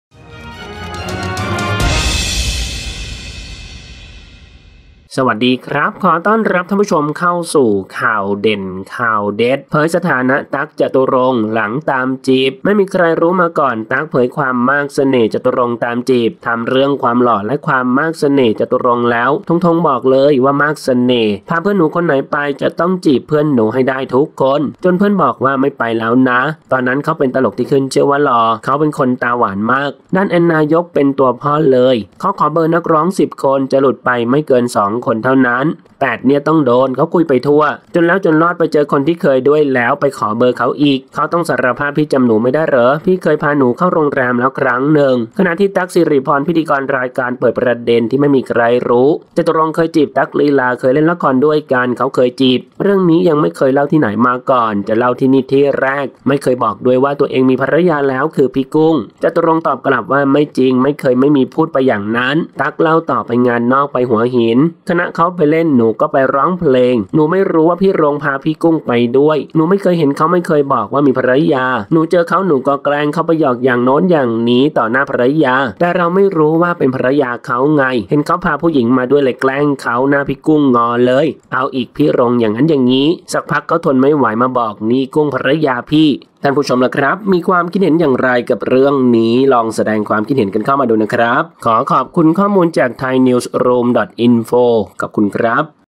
เผยสถานะ 'ตั๊ก-จตุรค์' หลังตามจีบ
ไม่มีใครรู้มาก่อน ตั๊ก เผยความมากเสน่ห์ จตุรงค์ ตามจีบ ถามเรื่องความหล่อและความมากเสน่ห์

ของจตุรงค์แล้ว ธงธง บอกเลยว่า มากเสน่ห์ พาเพื่อนหนูคนไหนไปจะต้องจีบเพื่อนหนูให้ได้ทุกคน

จนเพื่อนบอกว่าไม่ไปแล้วนะ ตอนนั้นเขาเป็นตลกที่ขึ้นชื่อว่าหล่อ เขาเป็นคนตาหวานมากด้าน แอนนา

ยกเป็ว่าตัวพ่อเลย เขาขอเบอร์นักร้อง 10 คน จะหลุดไปไม่เกิน 2 เท่านั้น 8 เนี่ยต้องโดนเขา

คุยไปทั่วจนแล้วจนรอดไปเจอคนที่เคยด้วยแล้ว ไปขอเบอร์เขาอีก เขาต้นองสารภาพพี่จำหนูไม่ได้เหรอ

พี่เคยพาหนูเข้าโรงแรมแล้วครั้งหนึ่ง ขณะที่ ตั๊ก ศิริพร พิธีกรรายการ เปิดประเด็นที่ไม่มีใครรู้ จตุรงค์

เคยจีบ ตั๊ก ลีลา เคยเล่นละครด้วยกัน เขาเคยจีบ เรื่องนี้ยังไม่เคยเล่าที่ไหนมาก่อนจะเล่าที่นี่ที่แรก

ไม่เคยบอกด้วยว่าตัวเองมีภรรยาแล้วคือพี่กุ้ง จตุรงค์ ตอบกลับว่า ไม่จริงไม่เคยไม่มีพูดไปอย่างนั้น ตั๊ก

เล่าต่อไปงานนอกไปหัวหิน คณะเขาไปเล่น หนูก็ไปร้องเพลง หนูไม่รู้ว่าพี่รงค์พาพี่กุ้งไปด้วย หนูไม่เคย

เห็นเขาไม่เคยบอกว่ามีภรรยา หนูเจอเขาหนูก็แกล้ง (เข้าไปหยอกอย่างโน้นอย่างนี้) ต่อหน้าภรรยาแต่

เราไม่รู้ว่าเป็นภรรยาเขาไง เห็นเขาพาผู้หญิงมาด้วยเลยแกล้งเขา หน้าพี่กุ้งงอเลย เอาอีกพี่รงค์ยังงั้นยังงี้

สักพักเขาทนไม่ไหวมาบอกนี่กุ้งภรรยาพี่ แม้ว่าหลังจากนั้นตั๊กและนุ้ยได้เป็นสามีภรรยากัน แต่ก็เกือบ

บ้านแตก “นุ้ย เชิญยิ้ม” เปิดใจ หลังถูกภรรยา “ตั๊ก ศิริพร” จับได้ว่าโอนตังค์กว่า 4 หมื่นบ.ให้

“ตลกสาว” นั่นก็คือ “ชมพู่ ก่อนบ่าย” นั่นเองค่ะ งานนี้สาวชมพู่จึงต้องออกมารีบชี้แจงทันทีว่า

“เป็นตังค์ที่เจ้าตัวจัดทริปไปเที่ยวสิงคโปร์ด้วยกัน 3 ครอบครัว ทุกคนที่จะไปร่วมทริปต้องโอนตังค์

มาที่บัญชีของชมพู่เอง เพราะ ต้องไปจองทัวร์ แล้วพอชมพู่ได้มาเจอกับพี่ตั๊ก พี่ตั๊กก็พูด บอกว่าเกือบบ้านแตก เตียงหัก ก็เพราะชื่อชมพู่นี่แหละ

ขอขอบคุณข้อมูลจาก: thainewsroom

00:00 - Intro
00:10 - News
01:00 - Minute